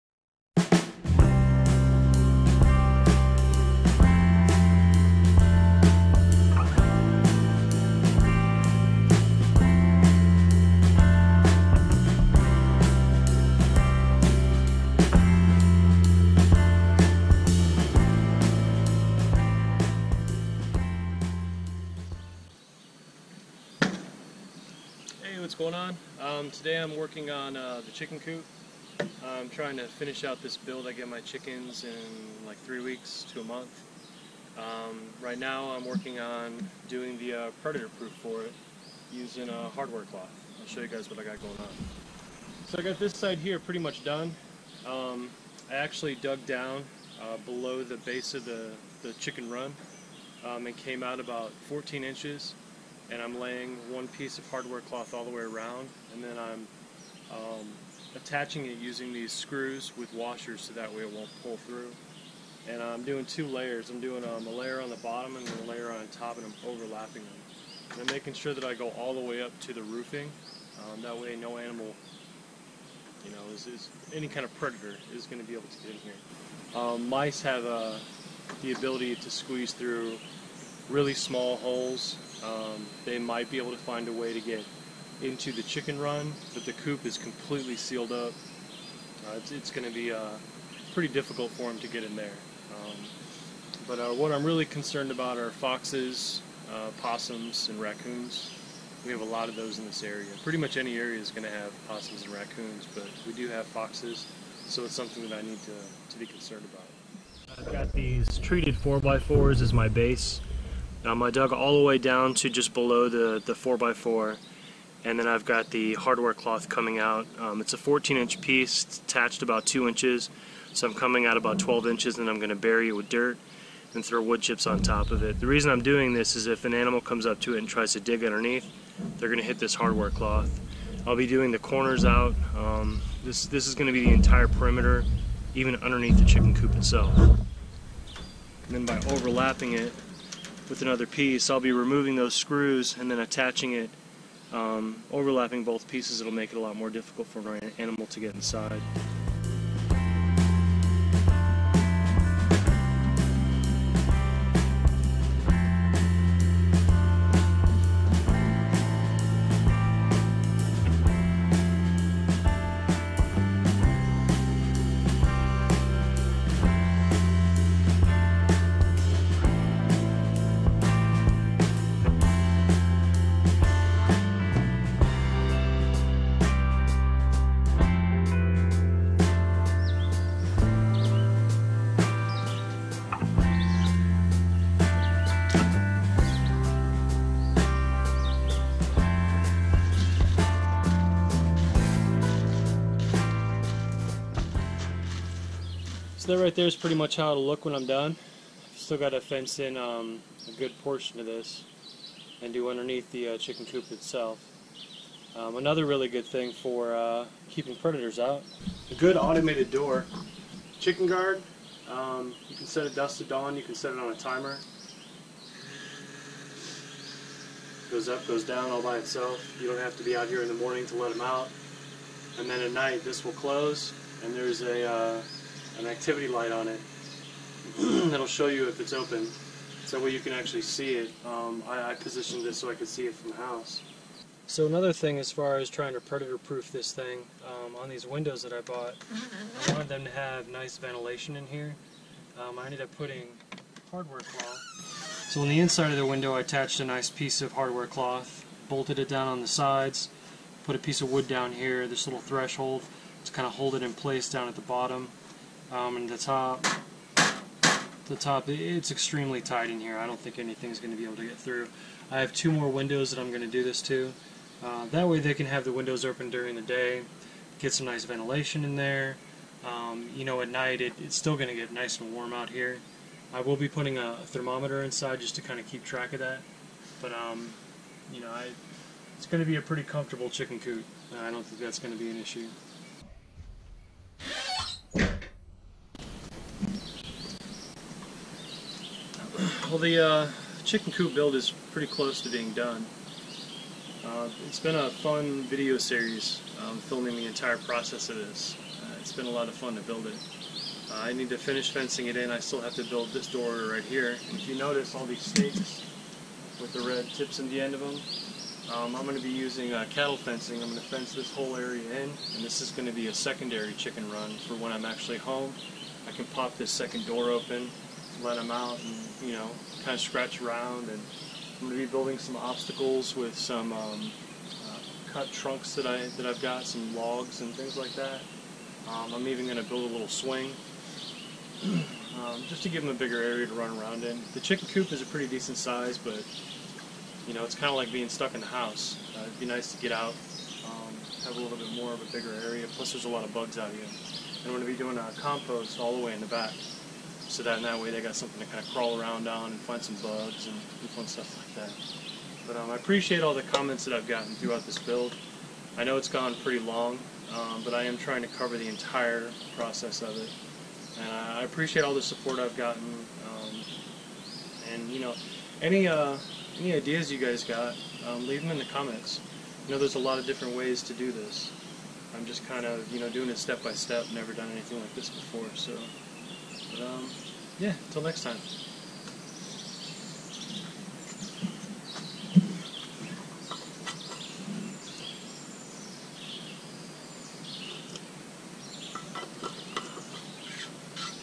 Predator proof the chicken coop!
Keeping predators out of the chicken coop is a necessity. Hardware cloth and a Chicken Guard automated door is a step in the right direction. This build is coming to a close. finally. Chickens coming soon!